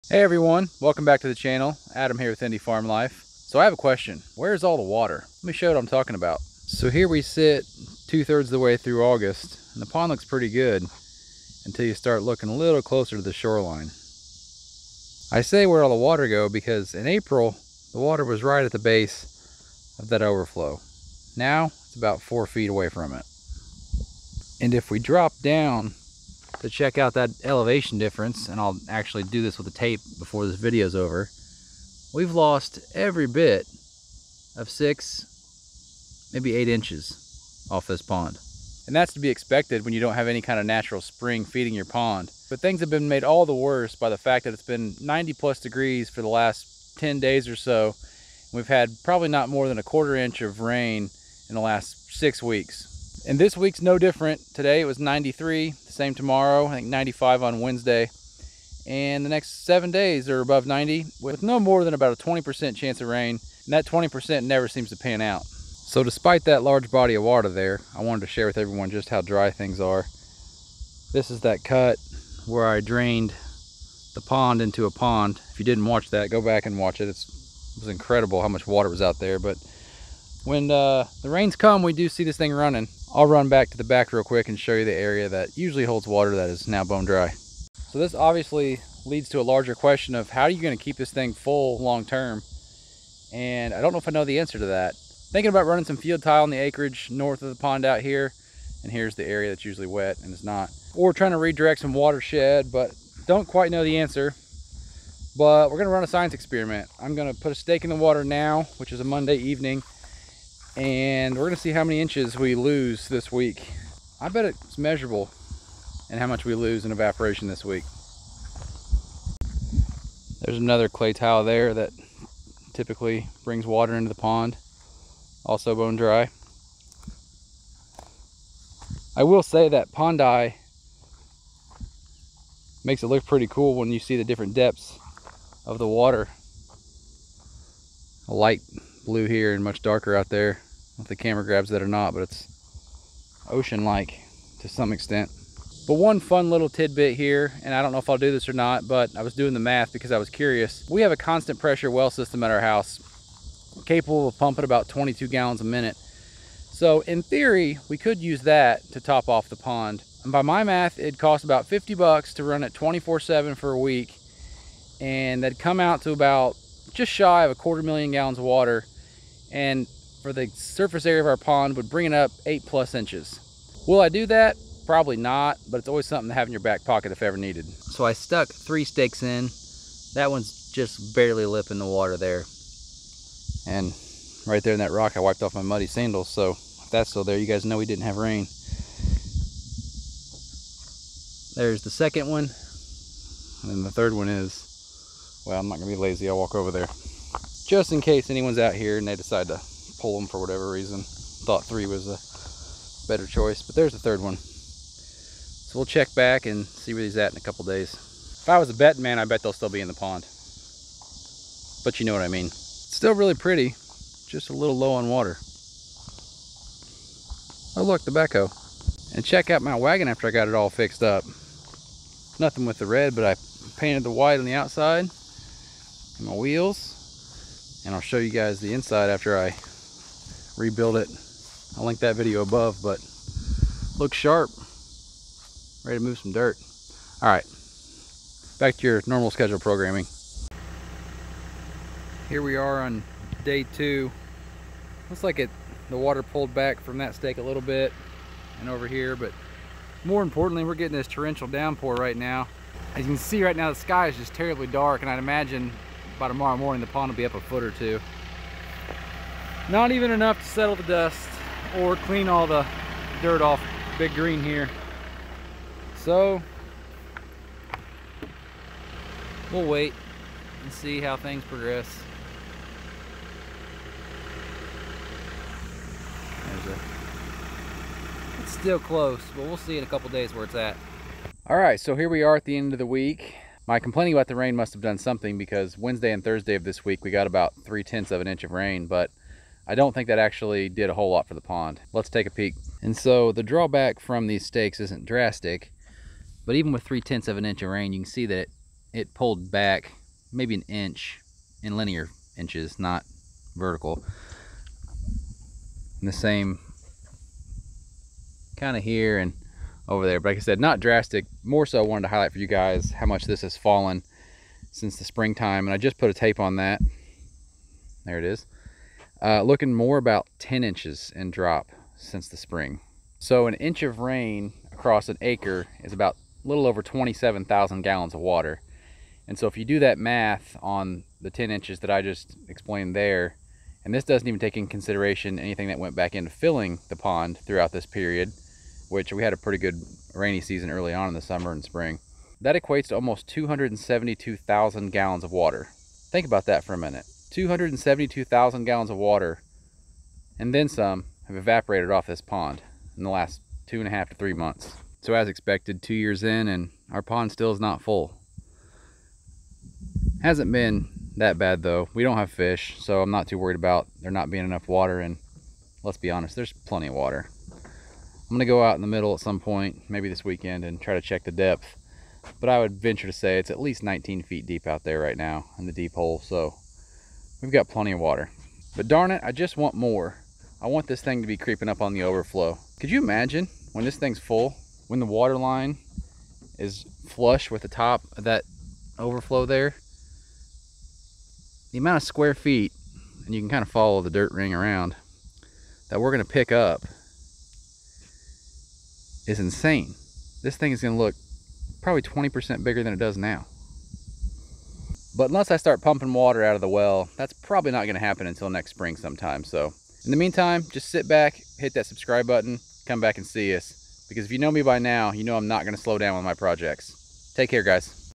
Pond Evaporation - How Much Water Did We Lose?
Evaporation on a pond is no joke!  How much do you think we lost to evaporation?  50k gallons, 100k gallons, more?  Come find out!

This storefront provides links to many of the products we use around the farm.